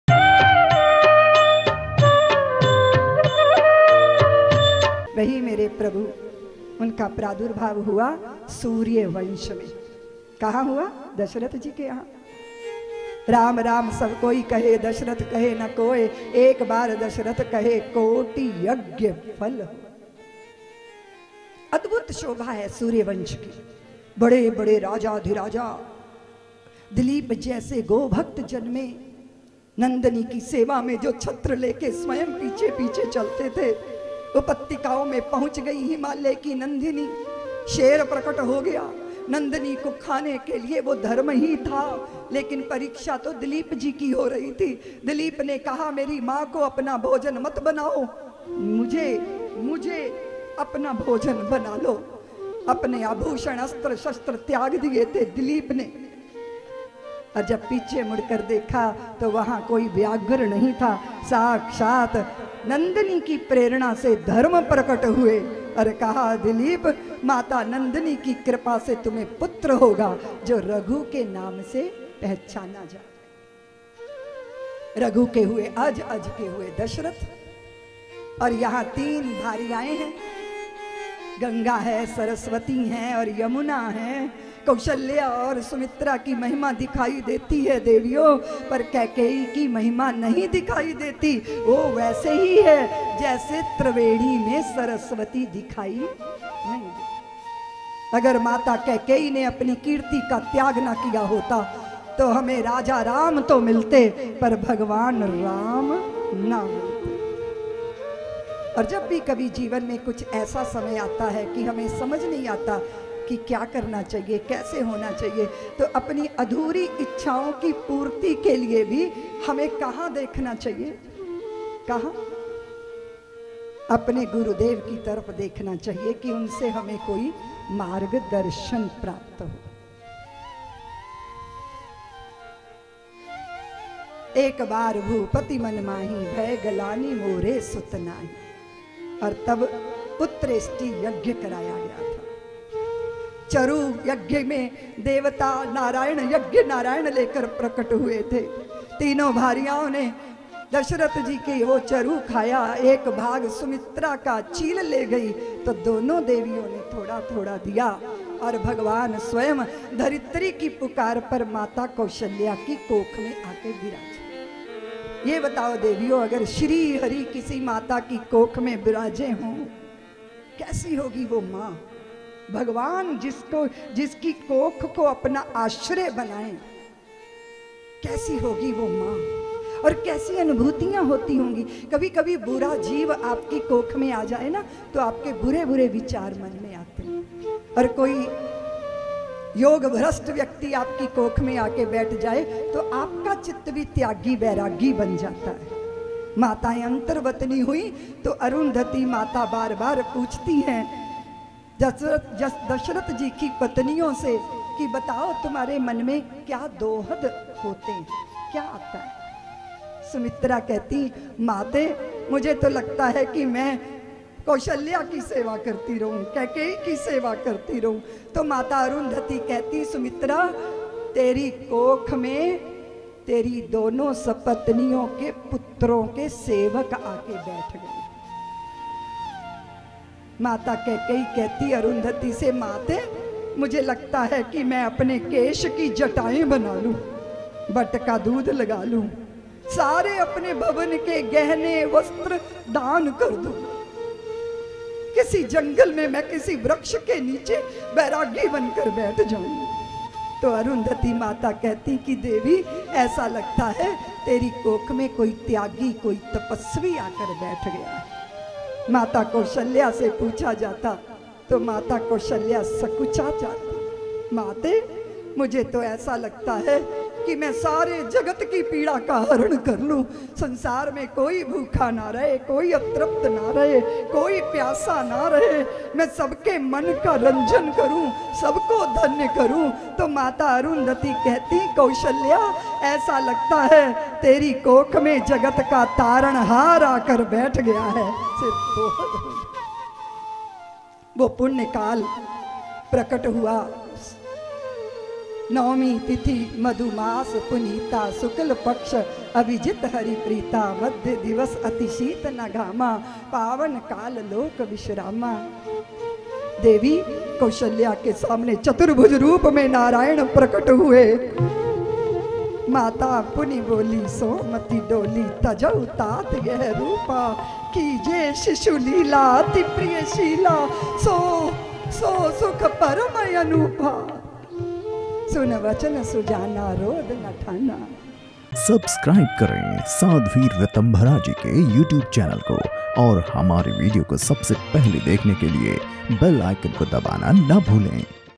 राम जन्म की कथा - Ram Janm Ki Katha - प्रभु श्री राम जन्म की कहानी - Sadhvi Ritambhara Ji
➤Video Name :- राम जन्म की कथा - Ram Janm Ki Katha
➤Artist Name :- Sadhvi Ritambhara Ji

➤Copyright :- Sadhvi Ritambhara Ji
❤ Social networking service :- Parveen Production House  M.No.9911079993 , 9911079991

Thank You So Much By :- Sadhvi Ritambhara Ji